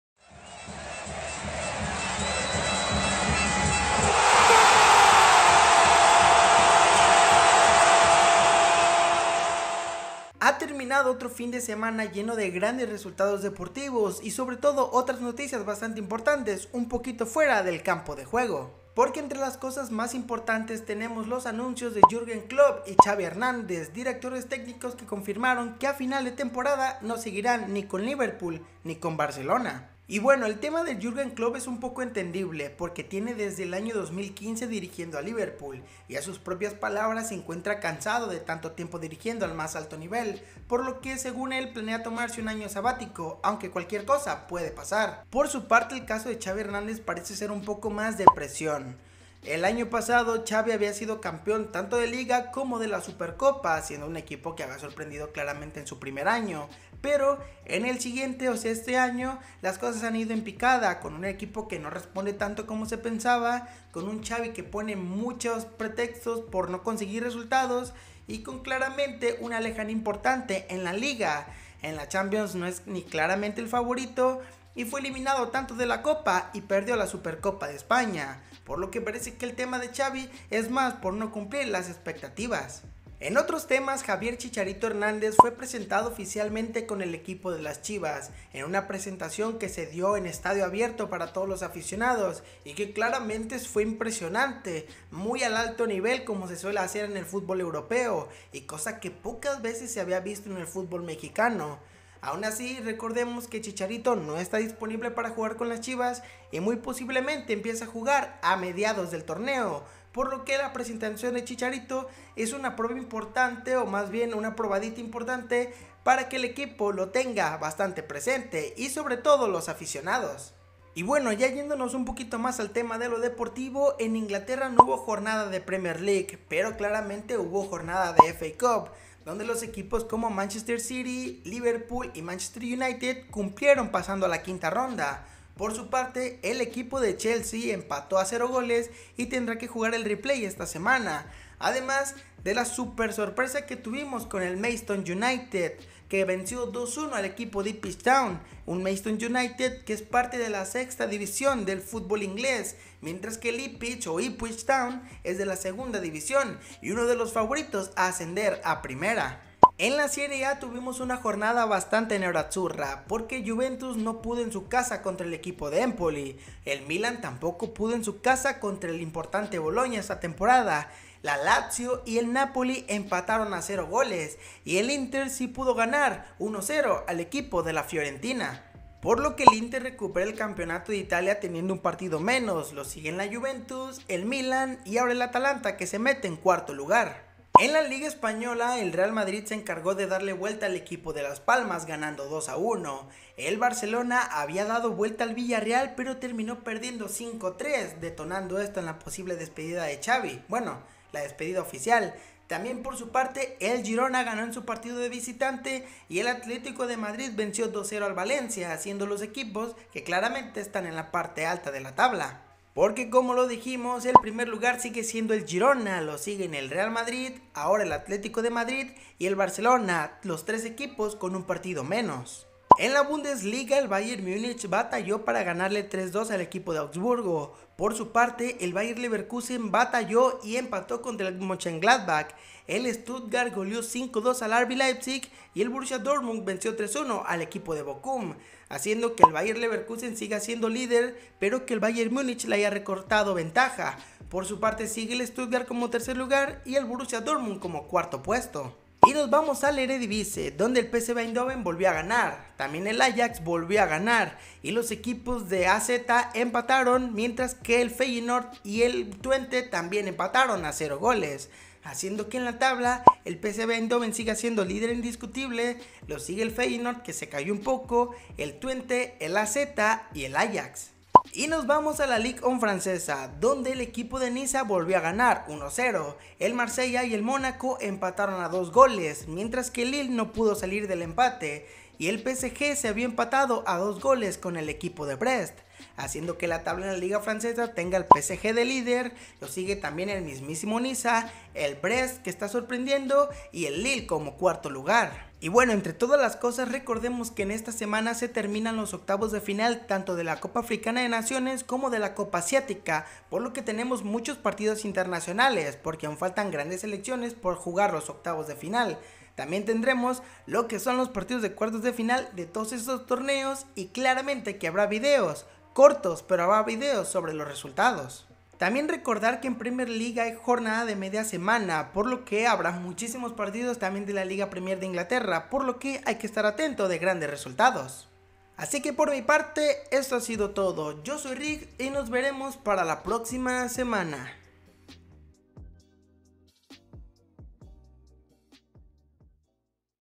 Klopp y Xavi se despiden | Presentación de Chicharito en Chivas | Resultados ligueros | Rsumen EF9
Otro fin de semana y otro resumen de TODO lo sucedido en el medio deportivo de europa (y en algunos casos de américa). Desde la despedida de Klopp y Xavi hasta el cambio de liderato en Italia.